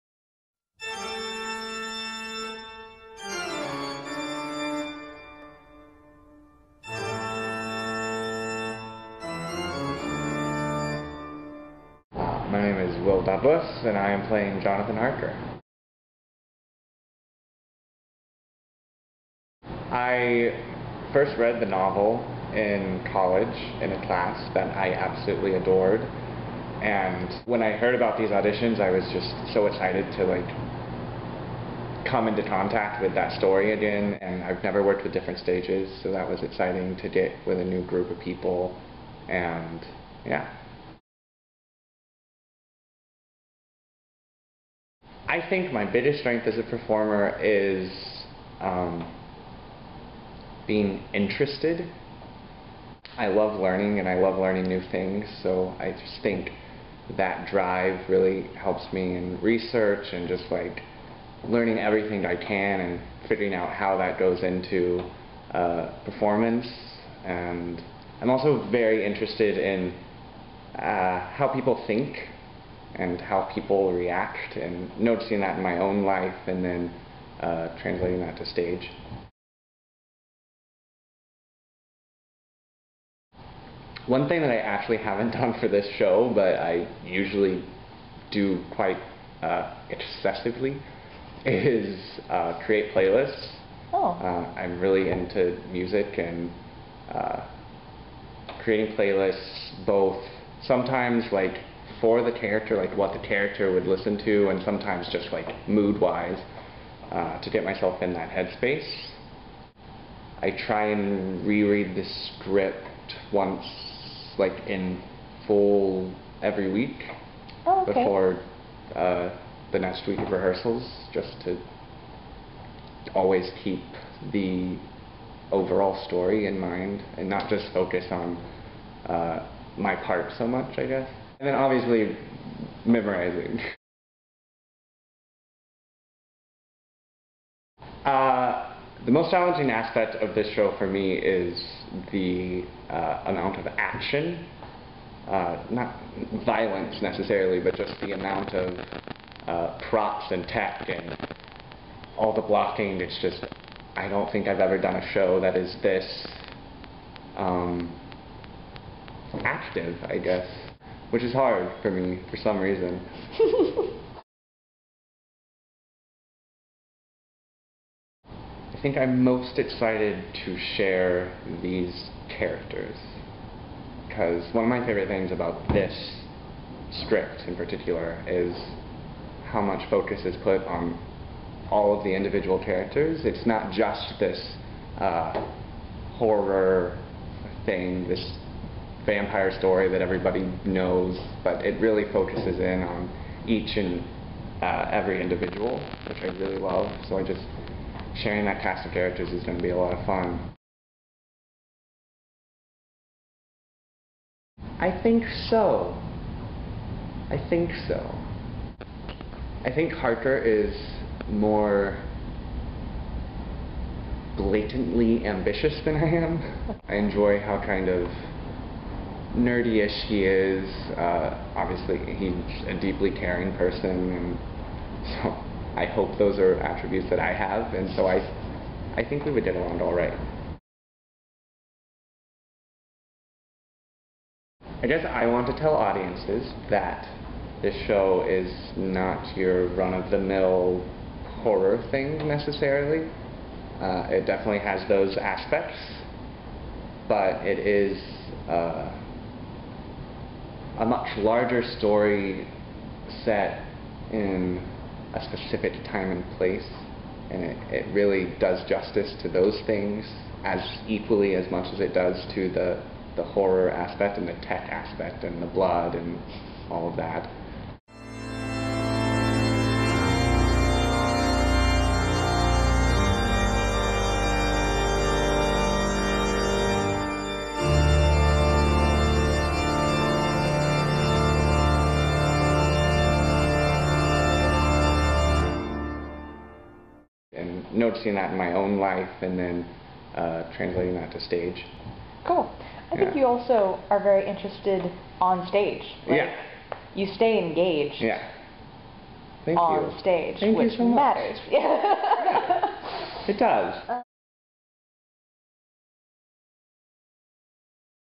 Dracula Interview #8 - Jonathan Harker: Will Douglas
Opening night of Different Stages' production of Dracula is just 3 days away!  Find out more about the show from cast member Will Douglas.
Photo credit: Errich Petersen

Dracula:
Nov 20th - Dec 12th
at the Vortex